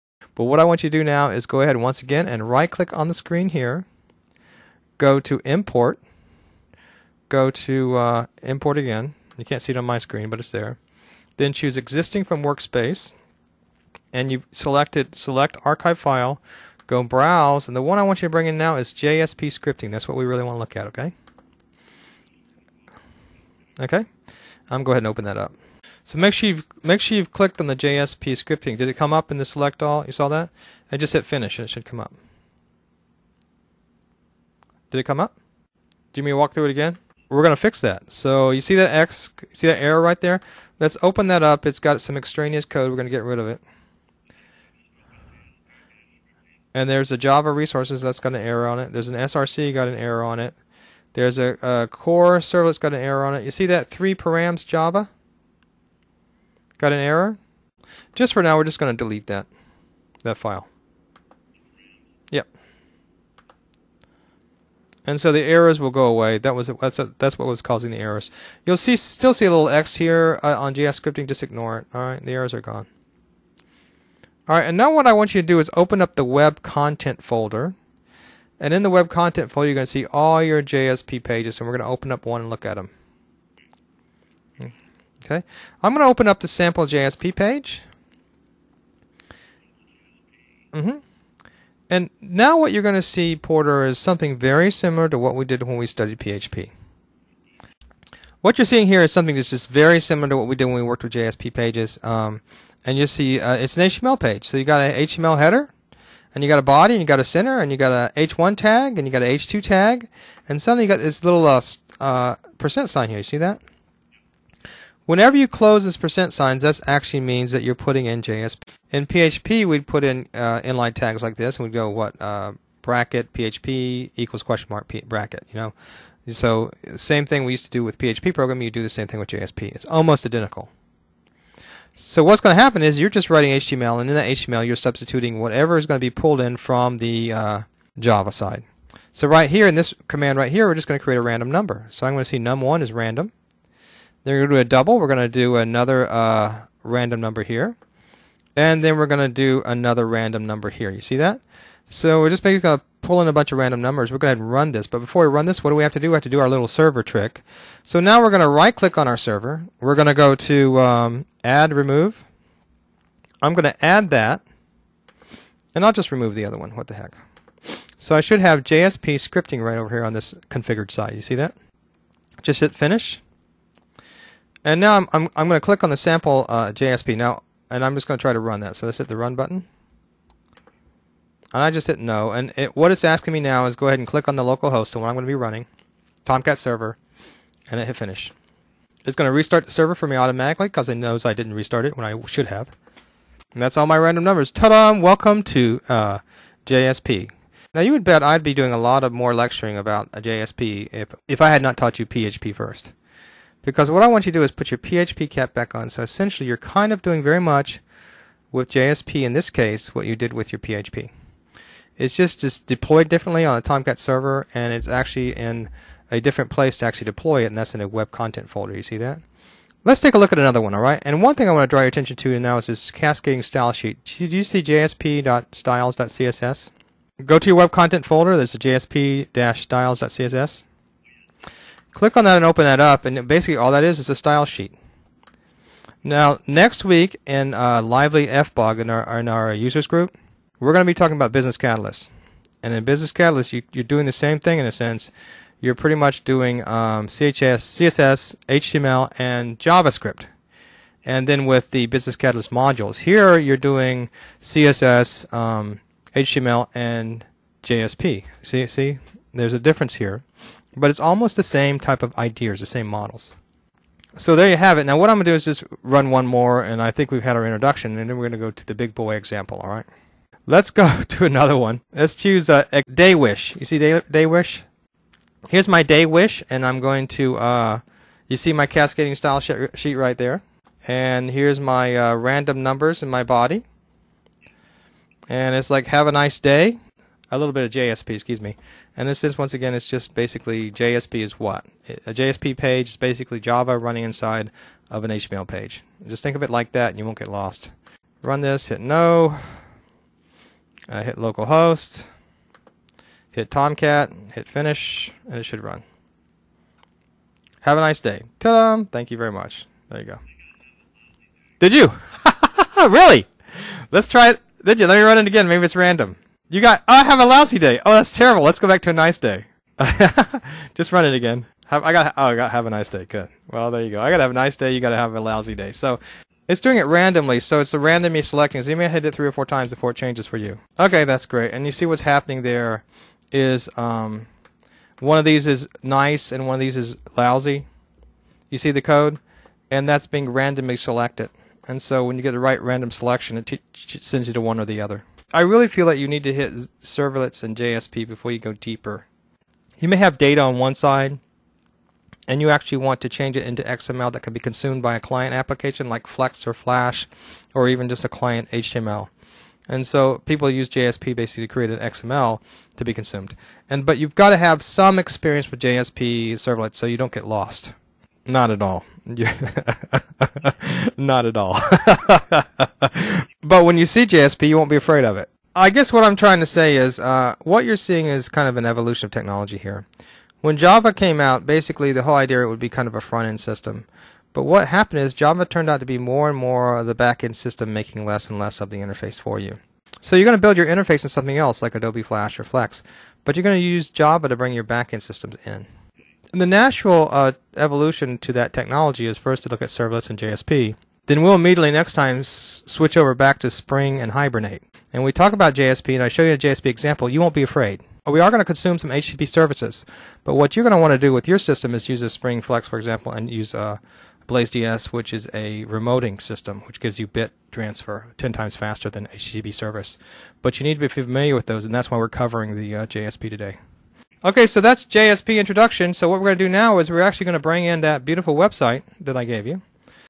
Java Programming 38 Running JSP Pages in Eclipse
In this tutorial you learn how to run JSP pages in Eclipse.